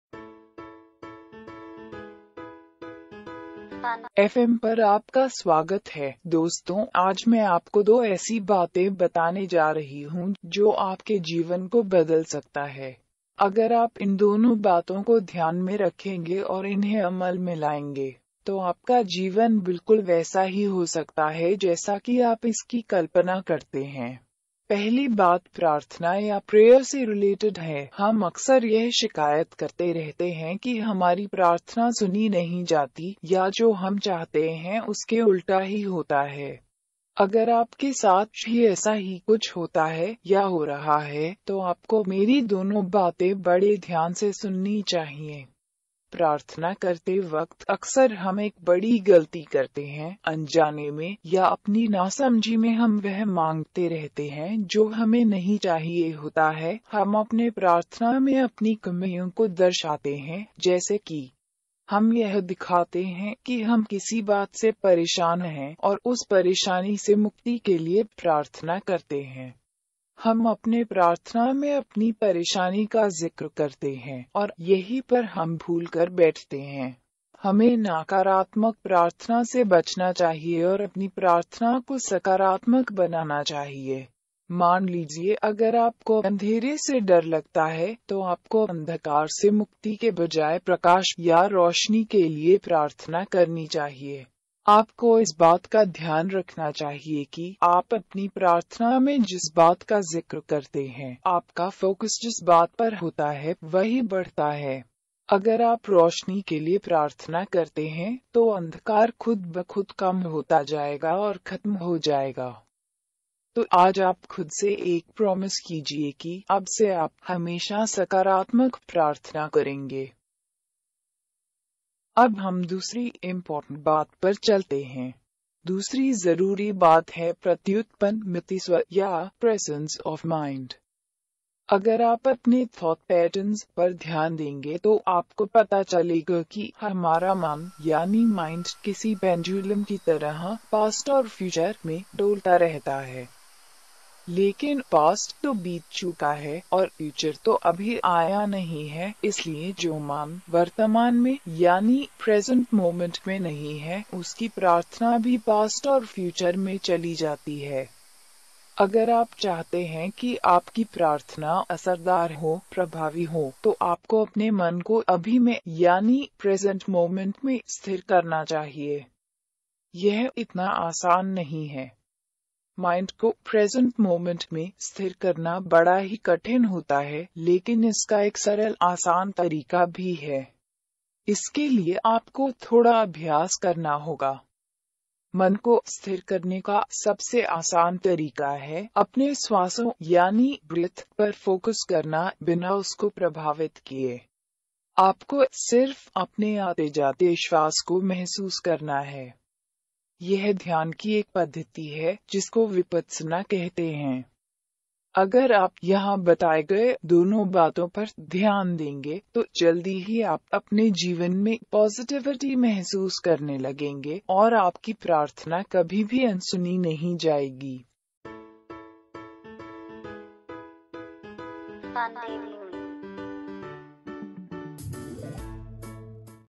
जानिए क्यों प्रार्थनाएँ नहीं सुनी जा रही हैं और अपने जीवन को सकारात्मक ऊर्ज़ा से भर दीजिए
अगर आपको लगता है की आपकी प्रार्थनाएँ नहीं सुनी जा रही हैं और आप जैसा चाहते हैं उसके उल्टा ही सब हो रहा है, तो आपको इस विडियो को ज़रूर देखना चाहिए। यहाँ आपको दो ऐसी प्रभावशाली जानकारियाँ दी जाएँगी जो आपकी ज़िन्दगी बदल सकती है।